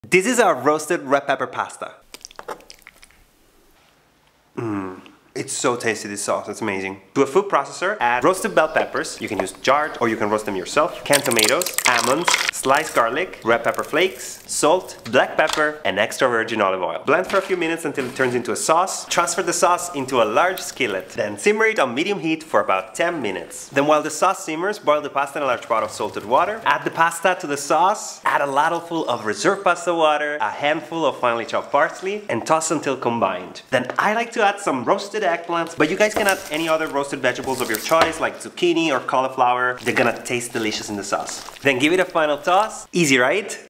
Roasted Red Pepper Pasta (30-min meal)
⭐️  Roasted red pepper pasta is one of the easiest and tastiest pasta dishes you can make with a few simple pantry staples.

Our recipe is simple, and you can make it with minimal effort for an everyday dinner. It's also creamy, nutritious, and delicious.

⭐️ Ingredients
RED PEPPER SAUCE
12 ounces (350 grams) roasted red bell peppers drained, from a jar or can
1 small can (15 oz) (400 grams) tomatoes whole peeled, diced, crushed, or puree
¾ cup (100 grams) almonds
2 tablespoons (28 grams) olive oil
1 teaspoon (5 grams) salt
1 clove garlic
½ teaspoon red pepper flakes

1 pound (450 grams) rigatoni pasta or other
1 handful parsley or basil

ROASTED VEGGIES (OPTIONAL)
1 large cauliflower or eggplant
1 tablespoon (14 grams) olive oil
½ teaspoon (3 grams) salt

❤️ Nico & Louise
Theplantbasedschool.com